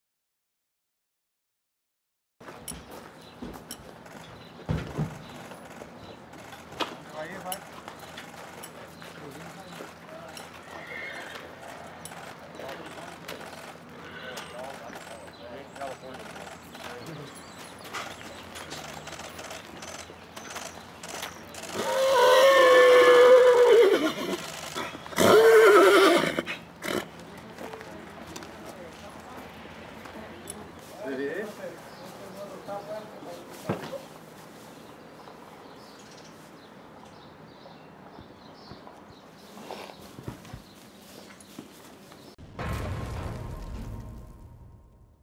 Rock Your World Arrives at Churchill Downs
Rock Your World arrives at Churchill Downs in Louisville, Kentucky for the Kentucky Derby.